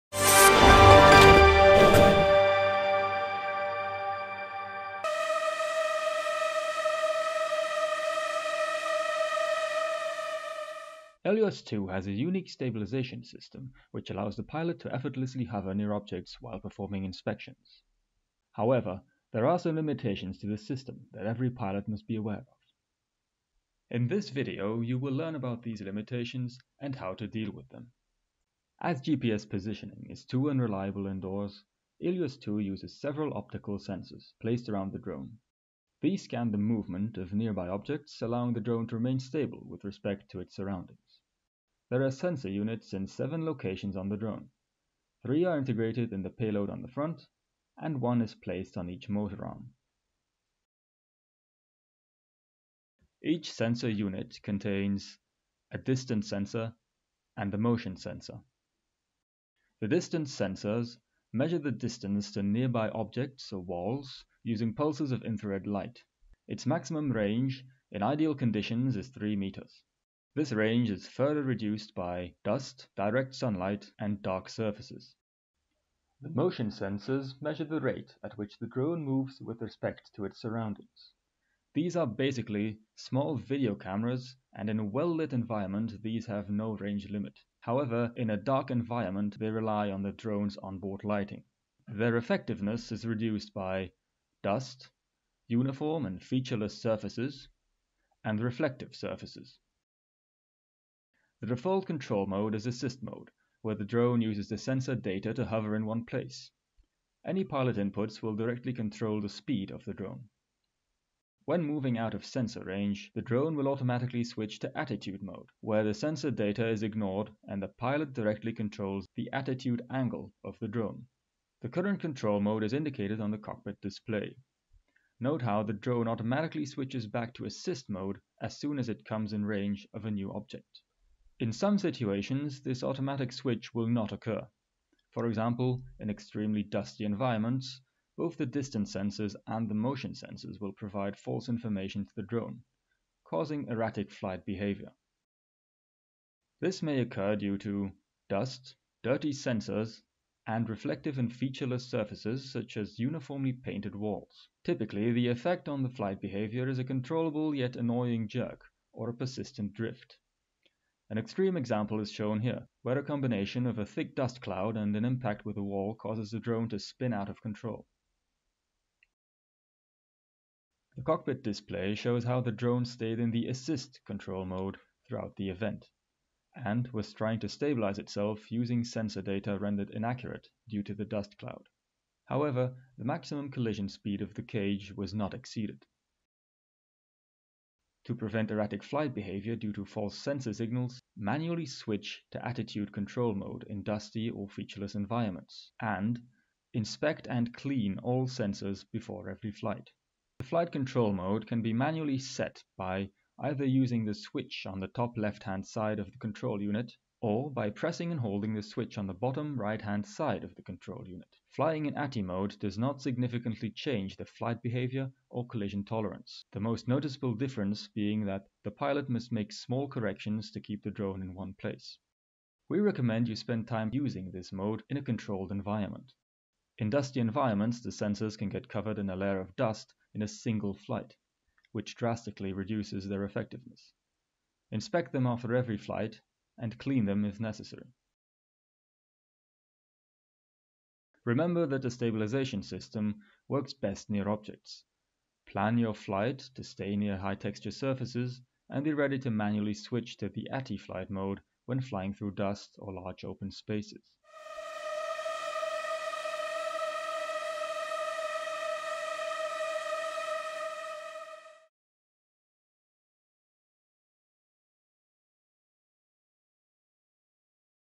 Flyability Stabilization Sensors
This video explains the methods used by Elios 2 to maintain stability in a GPS denied environment, and the limitations of this system.